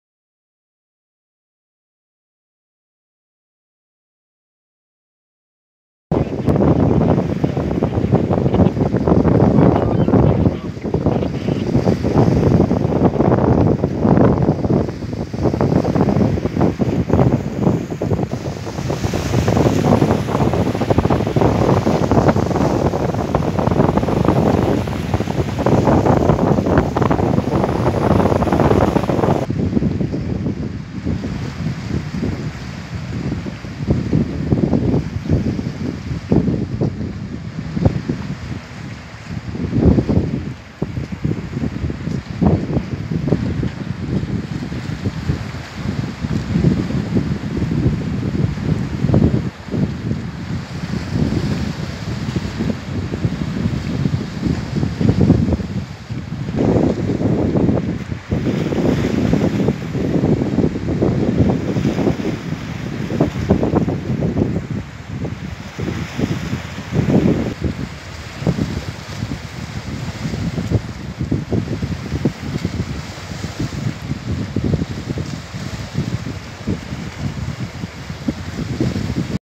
Maltepe'de, fırtına nedeniyle su almaya başlayan Ro-Ro gemisi battı
İstanbul'da etkili olan fırtına nedeniyle su almaya başlayan gemi battı.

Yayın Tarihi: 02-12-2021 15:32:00